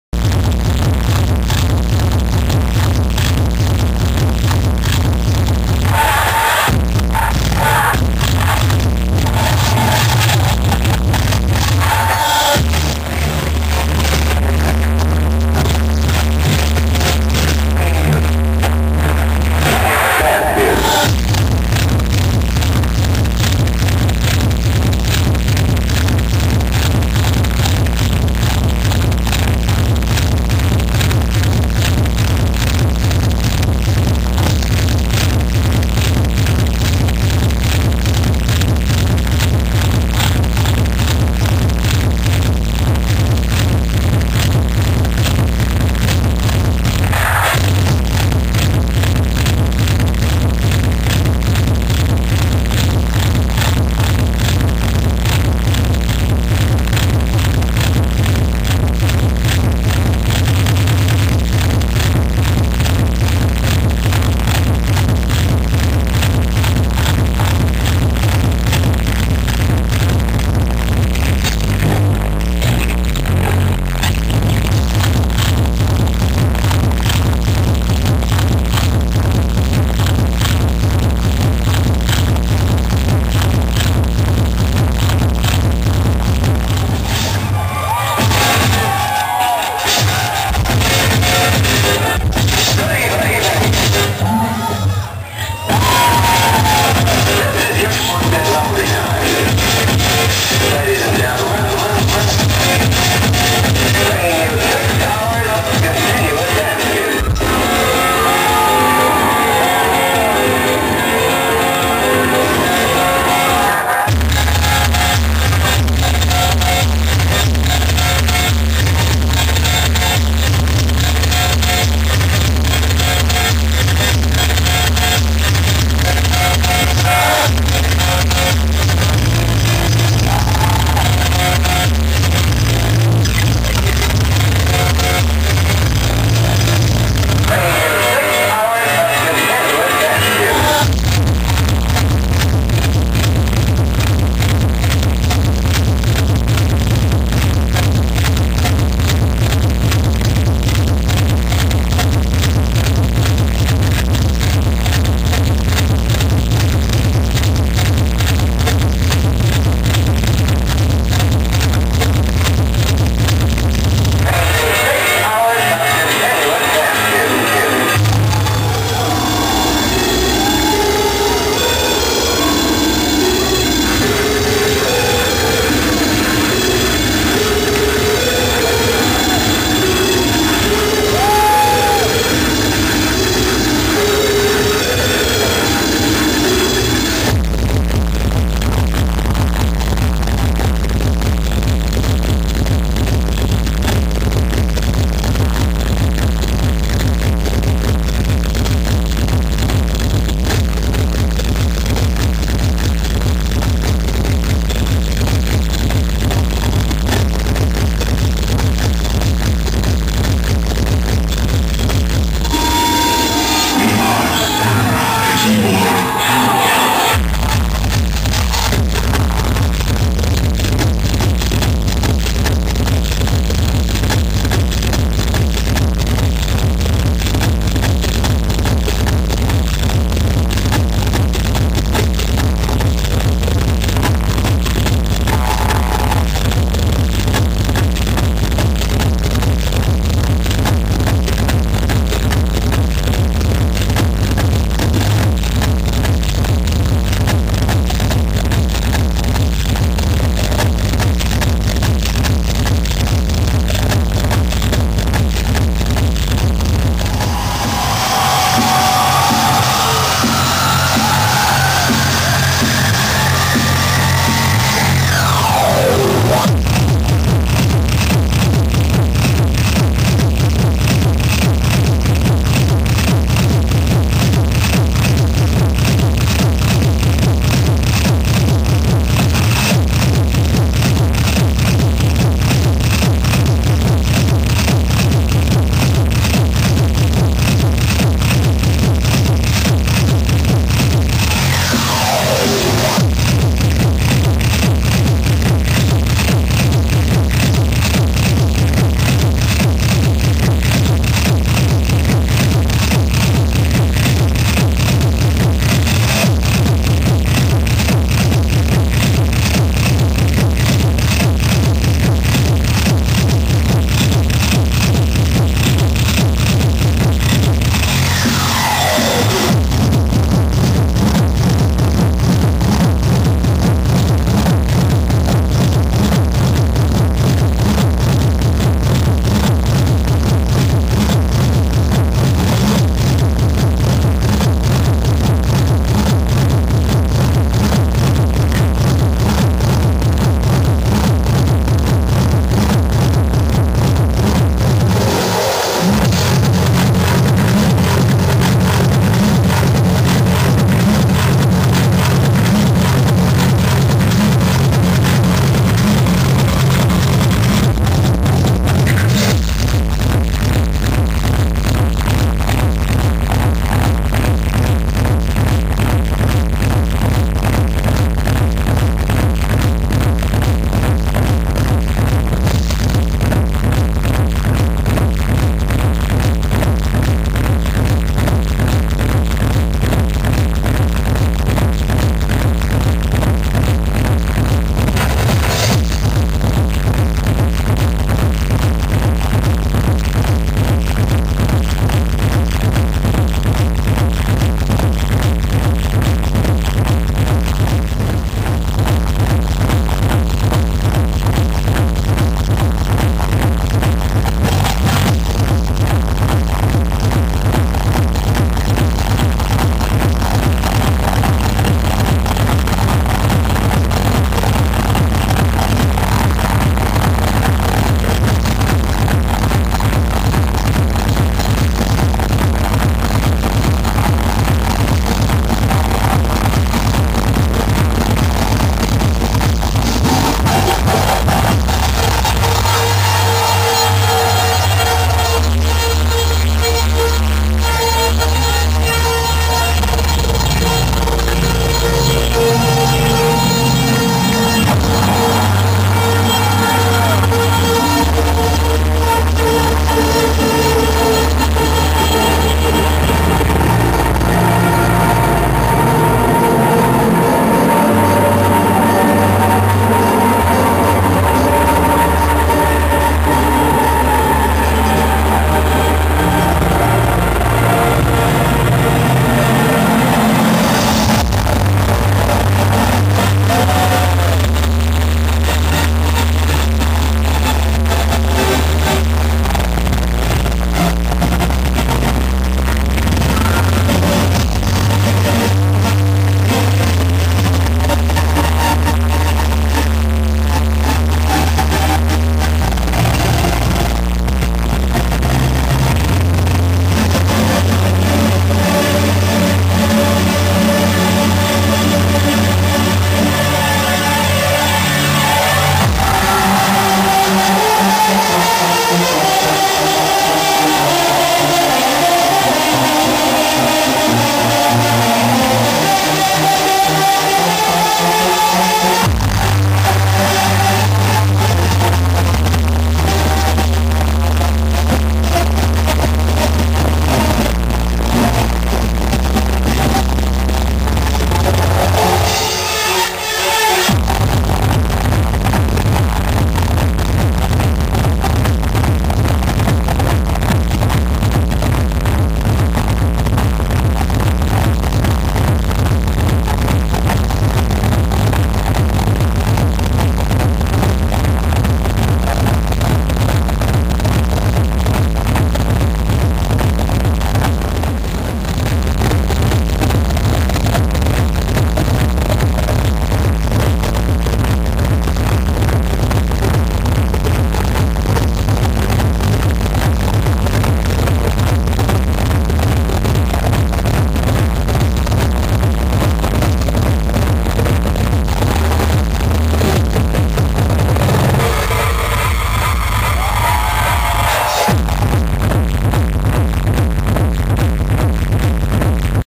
Yoji Biomehanika @ RED 080406
Medley of clips . . . sorry about the sound .. i think i was standing too close to the speakers :p